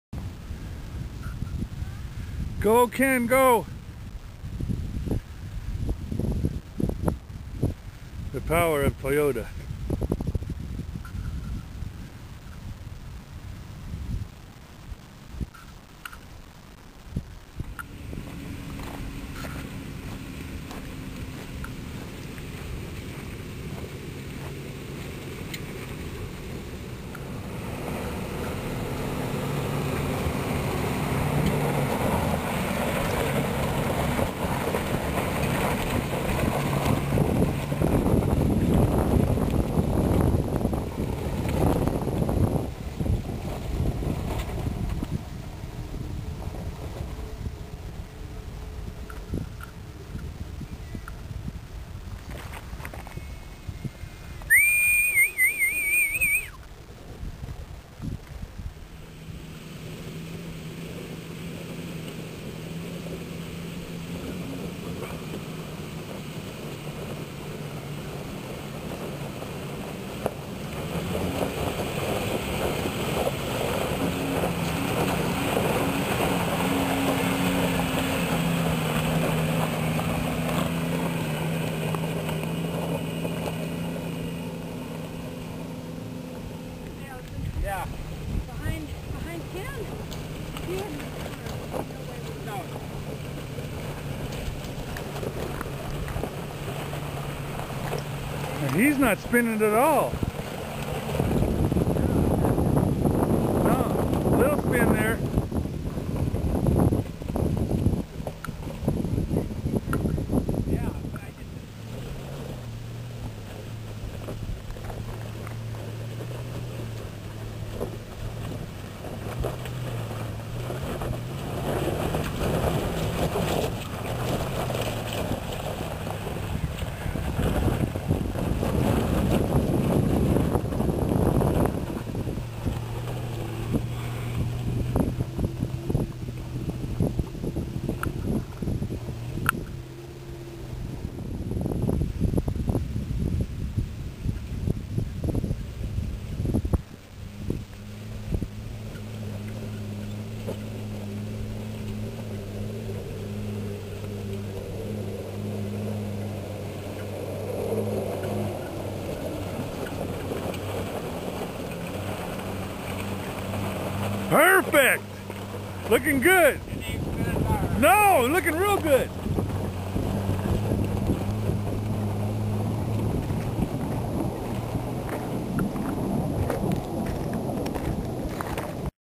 Ball Bearing Hill
DesertExplorers.org  ON a Chuckwalla Wilderness area trip.  This is on Dupont Road. We call it Ball Bearing Hill.